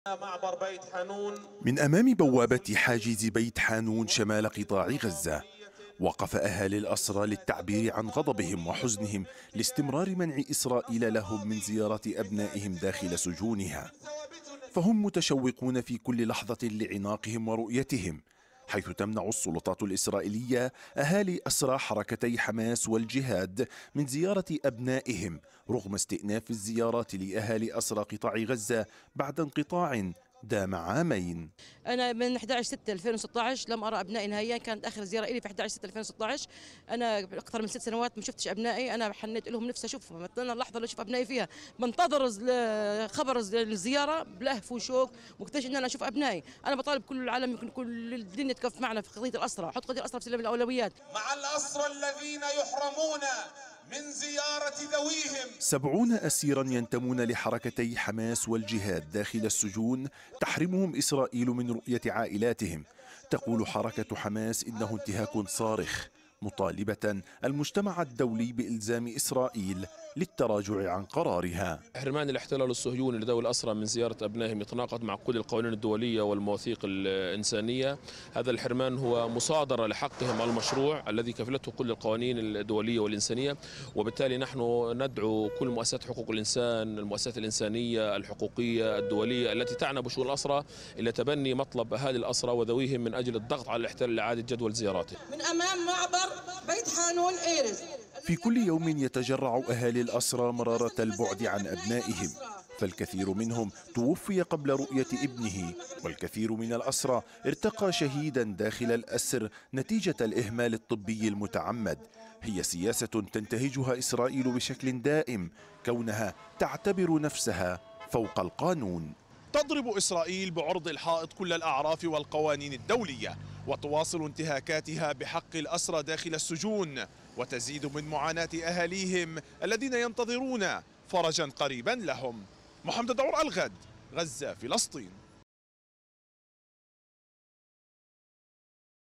أهالي الأسرى يطالبون بالضغط على إسرائيل للسماح لهم بزيارة أبنائهم داحل السجون
طالب أهالي أسرى حركتي حماس و الجهاد_الإسلامي المؤسسات الدولية والحقوقية بالتحرك الجاد للضغط على إسرائيل للسماح لهم بزيارة أبنائهم داخل السجون. حيث استأنفت إسرائيل جدول الزيارات لأهالي أسرى قطاع غزة بعد توقف دام عامين.
مزيد من التفاصيل في سياق تقرير مراسلنا في غزة محمد الداعور.